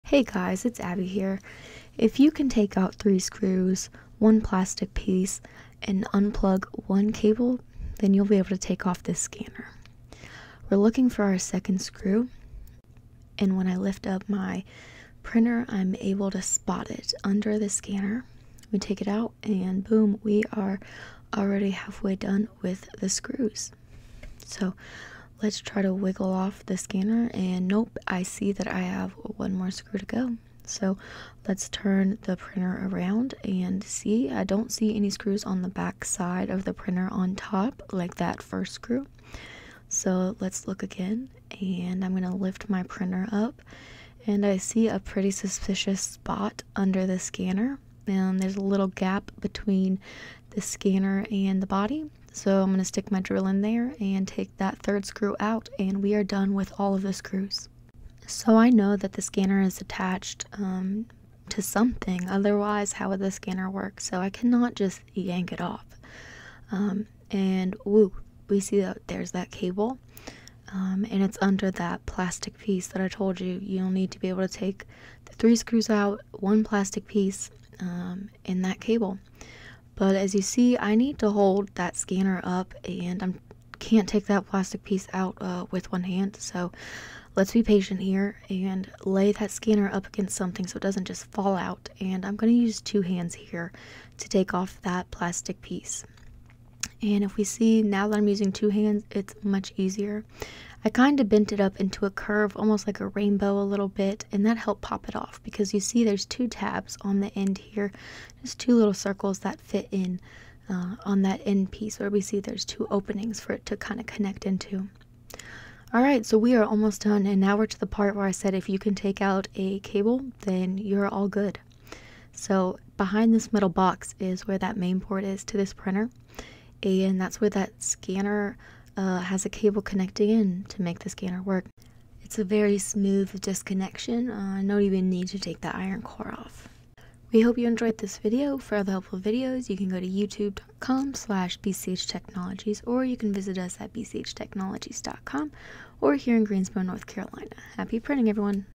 Unveiling the Secret: Easy Scanner Removal for Epson XP420
Hey everyone! Struggling with your Epson XP420 and need to remove the scanner? You're in the right place! Today, we'll walk you through the entire process step-by-step. It’s easier than you think – all you need is to remove a few screws, a plastic piece, and a cable. Stick with us as we show you how to tackle each part with ease, ensuring your printer maintenance is hassle-free. Dive into the world of DIY printer repair with BCH Technologies!

We will post the link to the whole YouTube video in the comment section.

This

 is a video about Epson XP420 How to Remove Scanner - Ready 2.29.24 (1)

0:00 Epson XP420 How to Remove Scanner - Ready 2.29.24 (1)